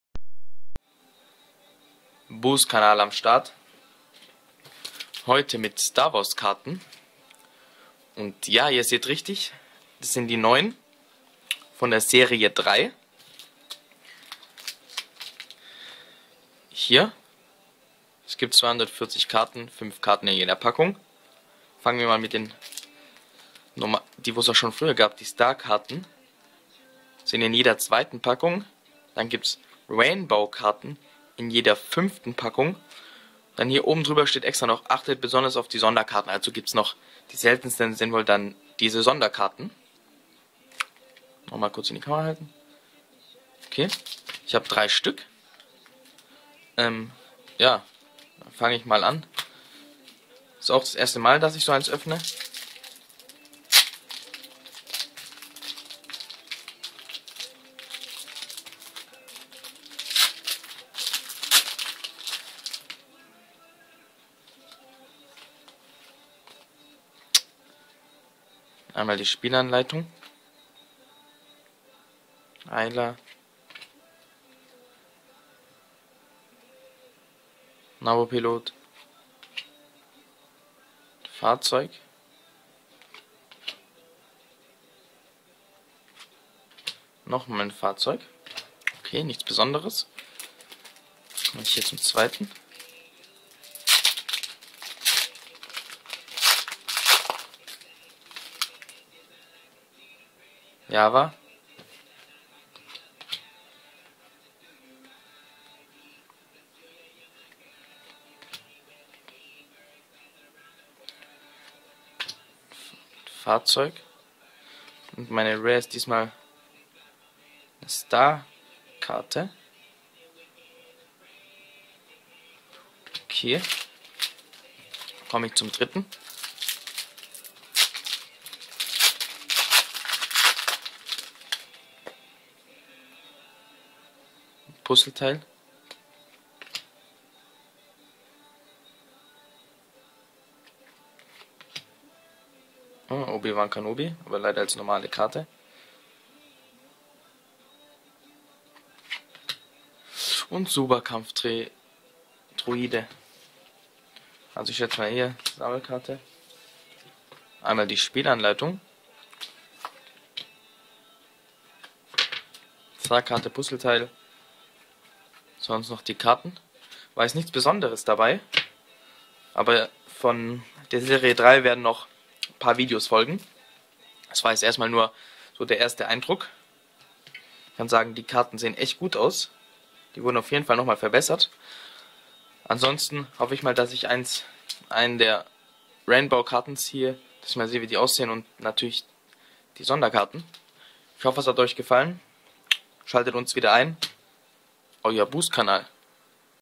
Force Attax Star Wars Serie 3 Karten 3 Booster Öffnen Opening NEU
3 Force Attax Star Wars Booster der Serie 3
Neu jetzt erhältlich
Viel Spass
(eventuell bald ein verkauf Video) haltet Ausschau.